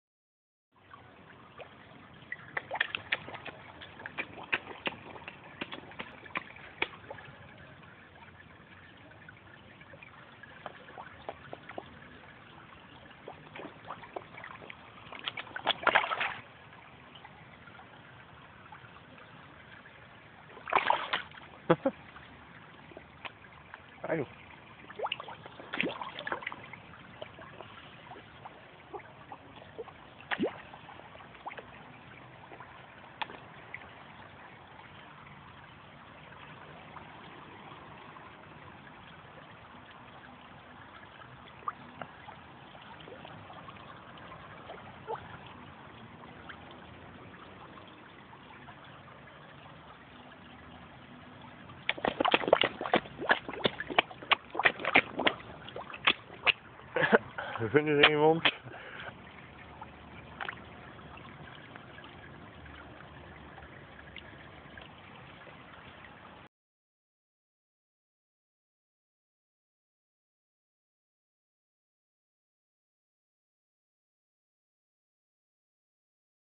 23 juli 2012 11:02
Having fun feeding my koi and sturgeons :)